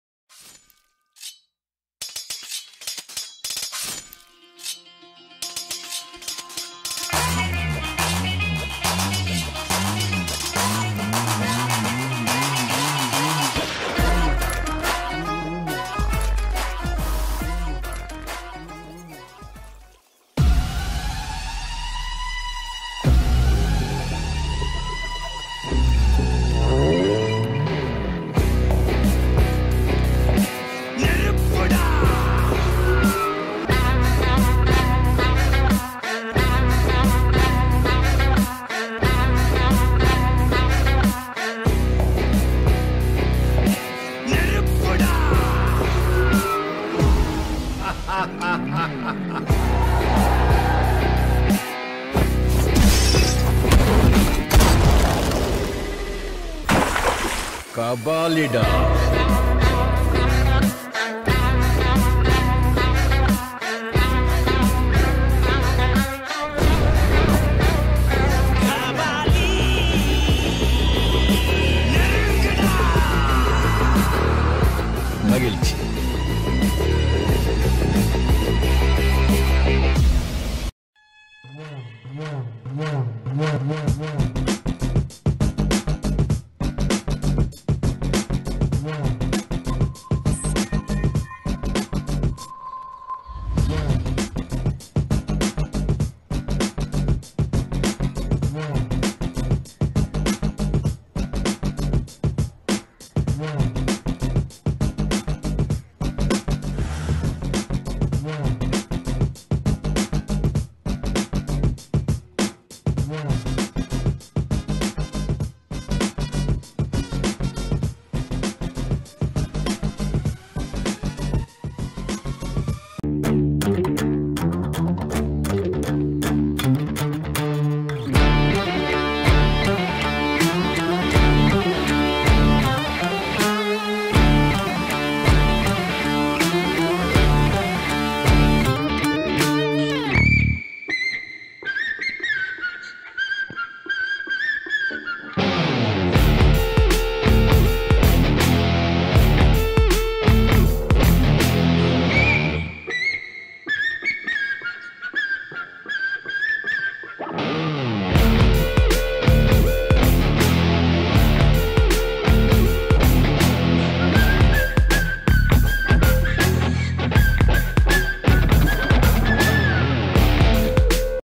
TEKNIK ASAS KAYAK SEC G1 KAABILASWARY
TEKNIK - TEKNIK ASAS BAGI SESIAPA YANG BARU MULA BERKAYAK.